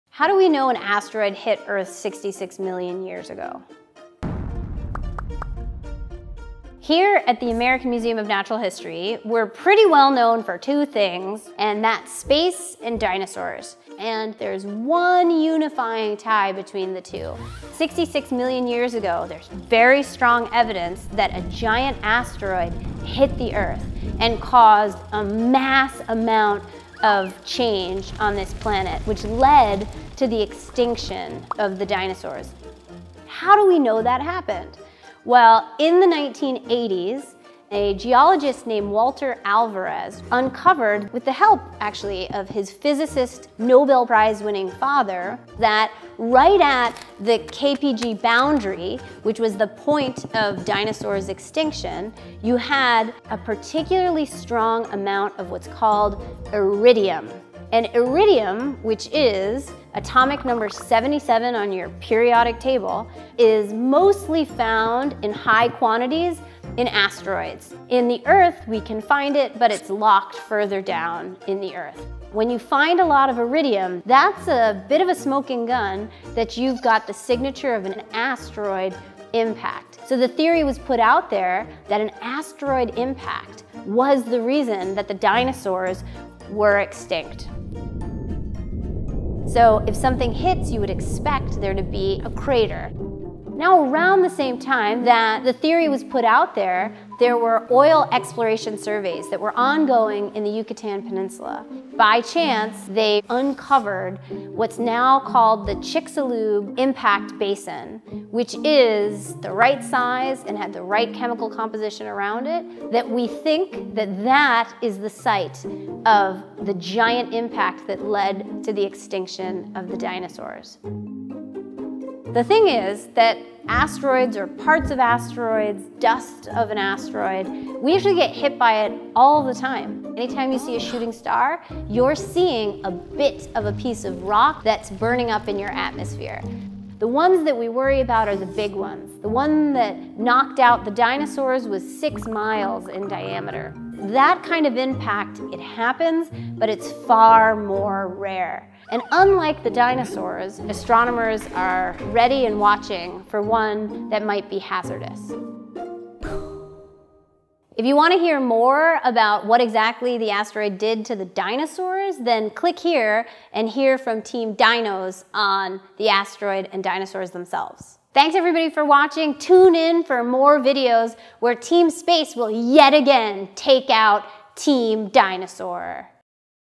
How do we know an asteroid hit Earth 66 million years ago?
One theory for why the dinosaurs went extinct is that an asteroid hit Earth at the end of the Cretaceous period. But since no one was alive to see it, how can we know that it really happened? Astrophysicist Jackie Faherty reveals how clues found in sedimentary rock and on the ocean floor have helped scientists piece together this millennia-old mystery.

If you want to learn more about the effect an asteroid impact would have had on life on Earth during the Cretaceous, watch this week’s Dinosaur video